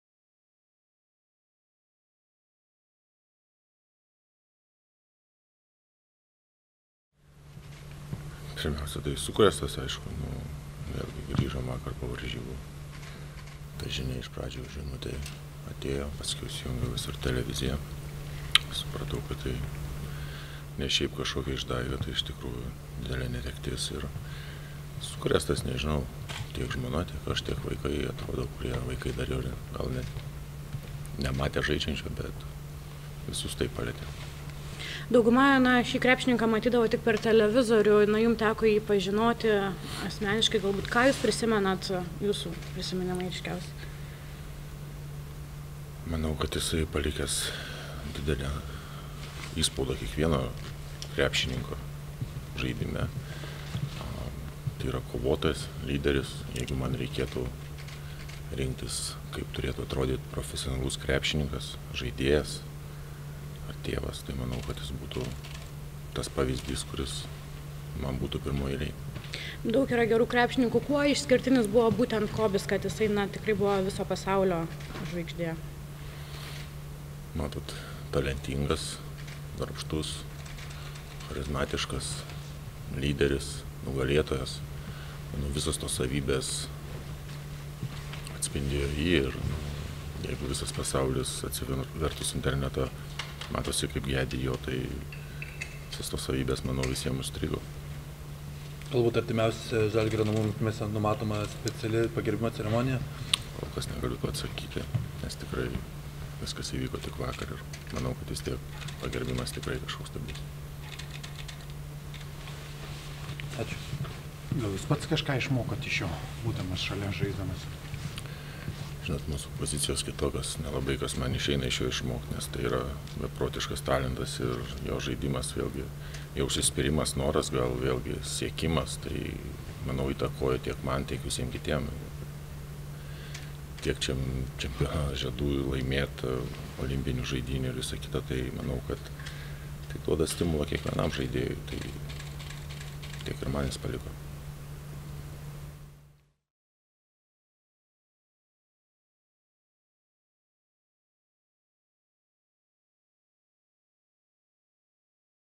R.Javtokas: „Kobe Bryantas – profesionalaus krepšininko pavyzdys“
Kauno „Žalgirio“ sporto direktorius Robertas Javtokas pasidalijo savo mintimis po pasaulį sukrėtusios žinios apie Kobe Bryanto netektį.